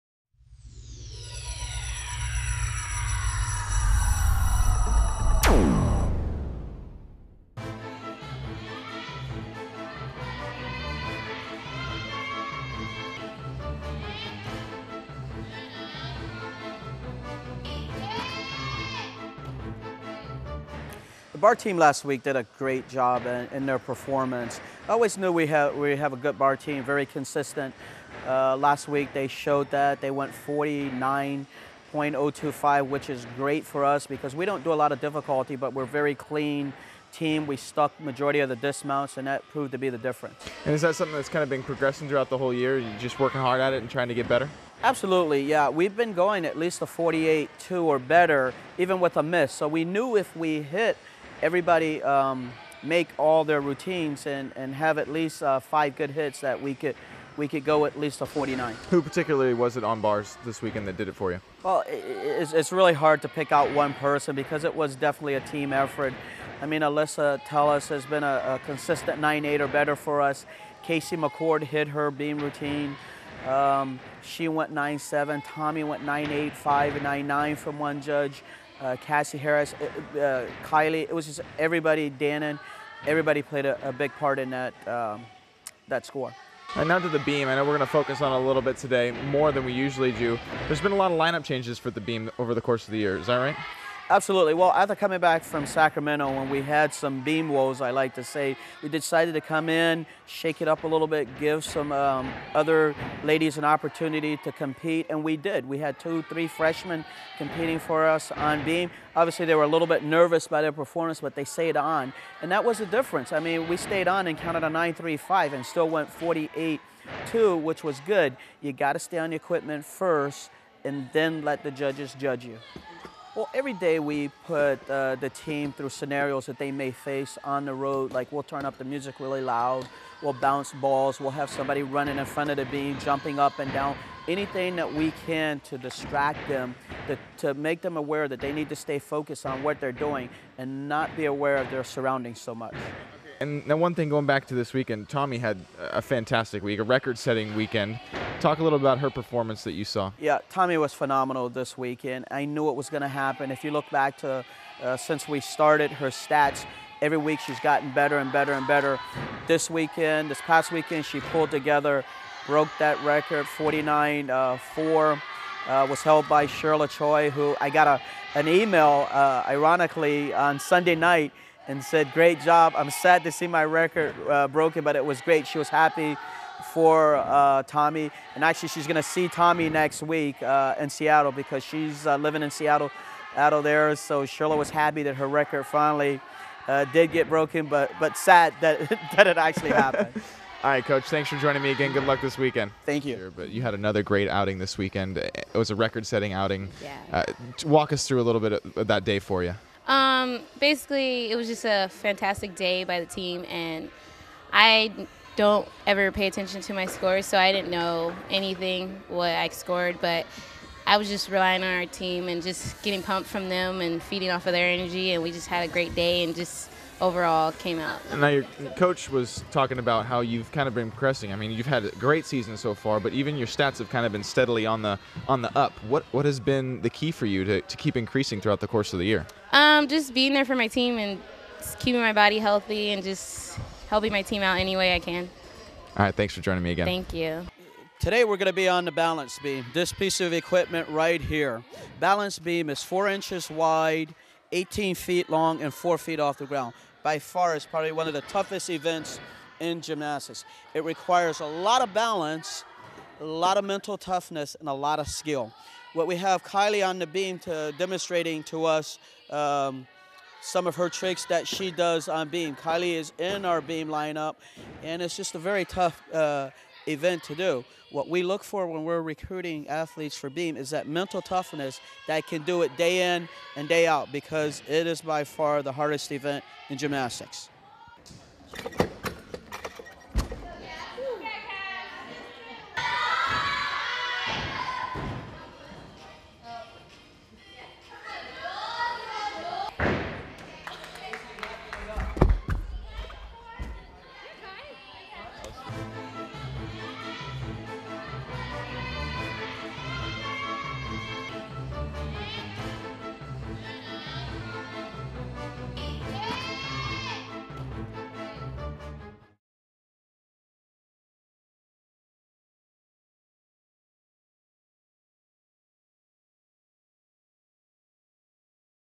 Gym Talk - February 8
Head coach Wayne Wright, with help from players, explains the proper technique to the Balance Beam.